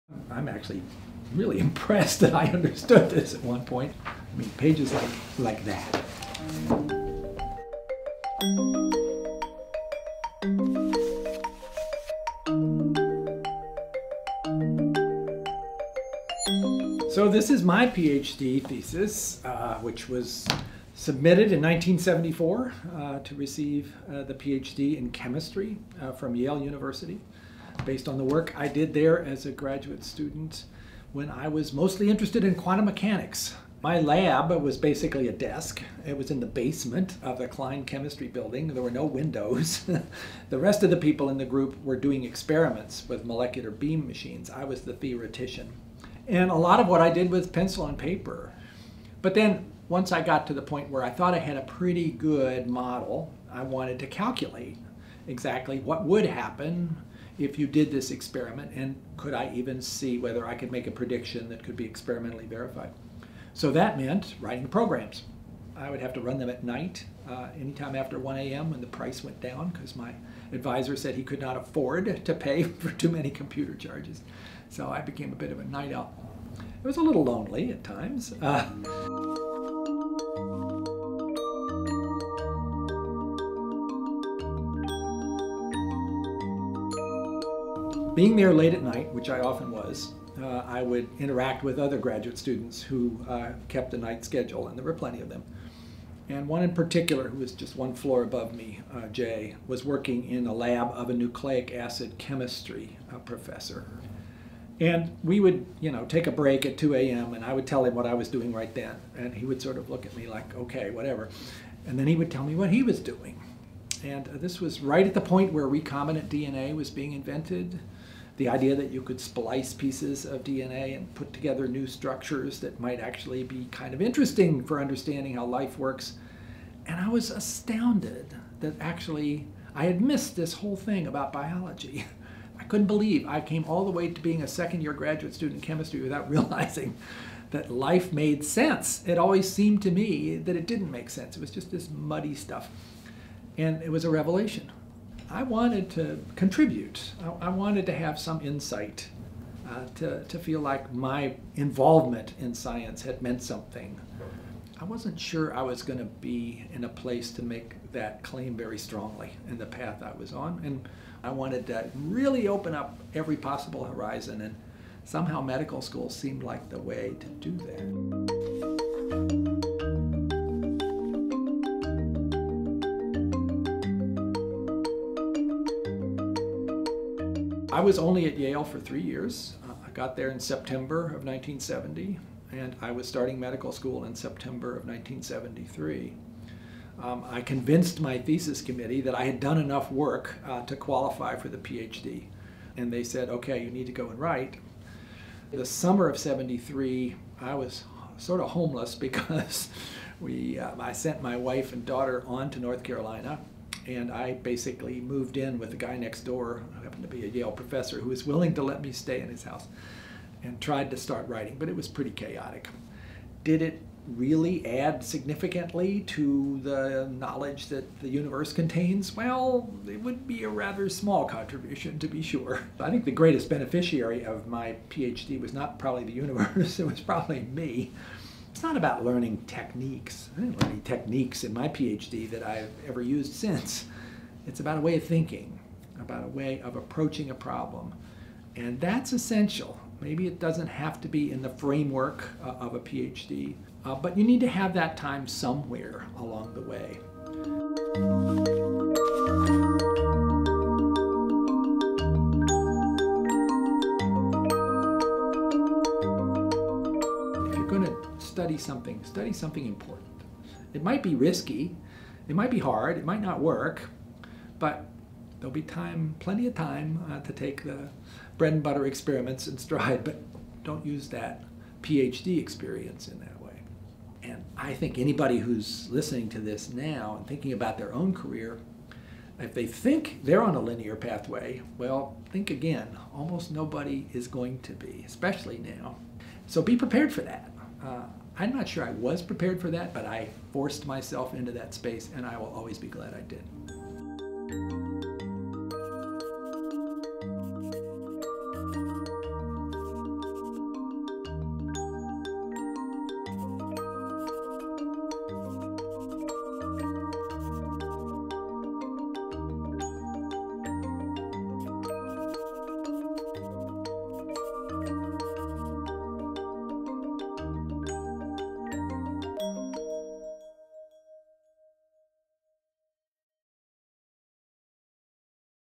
Back to the thesis: Francis Collins
In the first of this ‘Back to the thesis’ series from Nature Video, the director of the National Institutes of Health, Francis Collins, takes us back to the 1970s and his PhD - a time when his interests were less biomedical, and more quantum chemical.